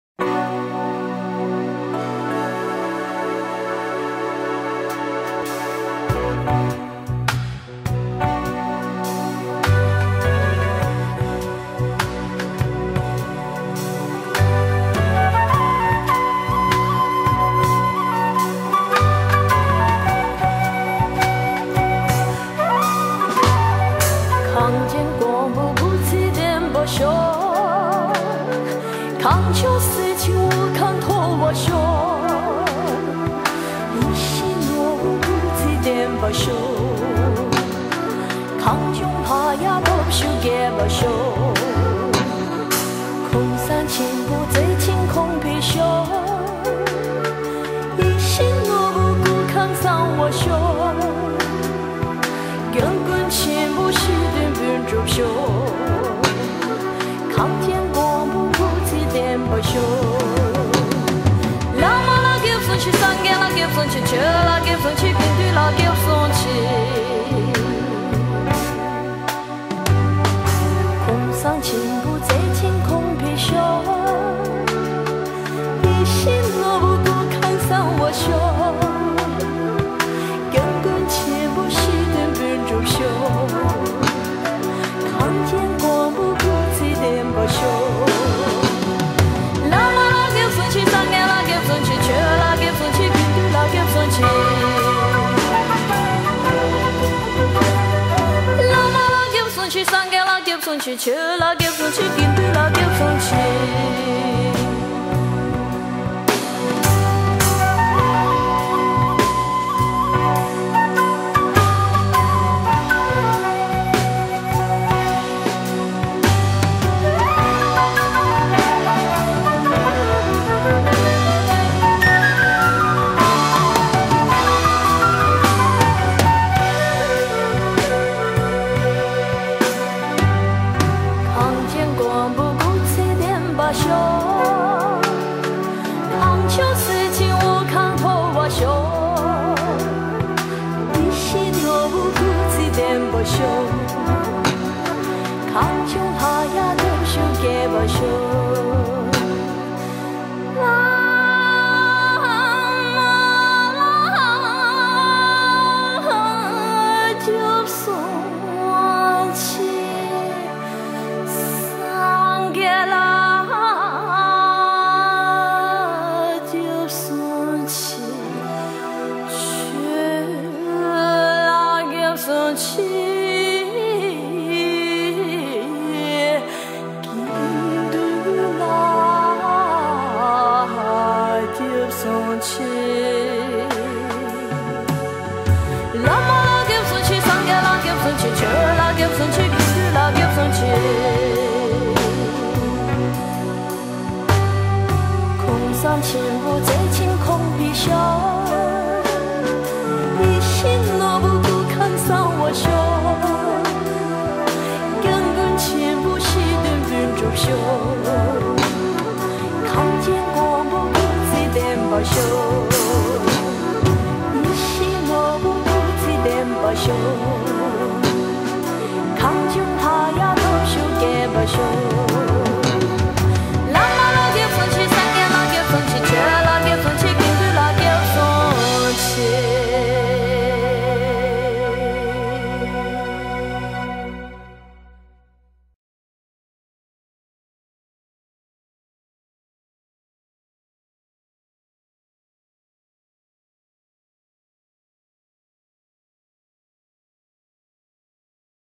Tibetan song - For His Holiness the Dalai Lama
Dedicated to the incomparable Chenrezig, His Holiness the Dalai Lama.